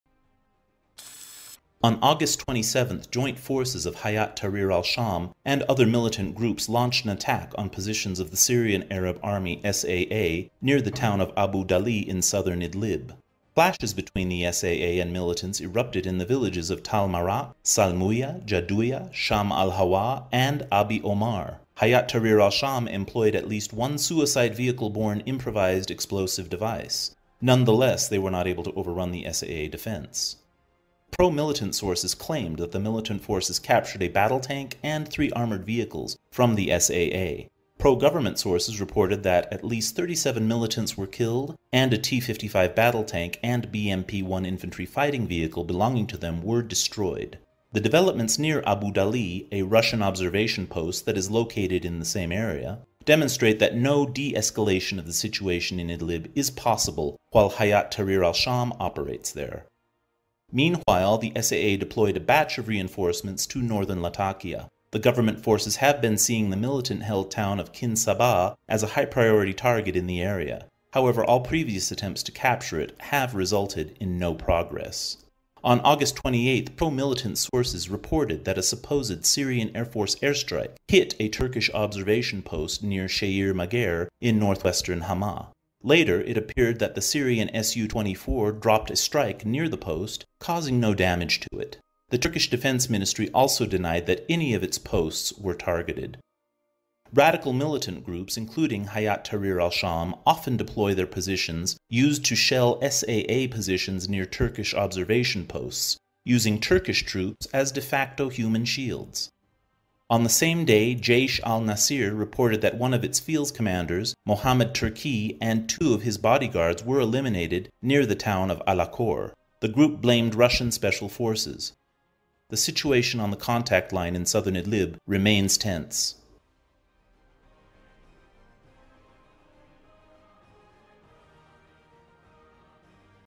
Idlib counter-terrorism update, 29 August 2019
HTS/Nusra attacks rebuffed in SE Idlib, little progress so far in NW Idlib.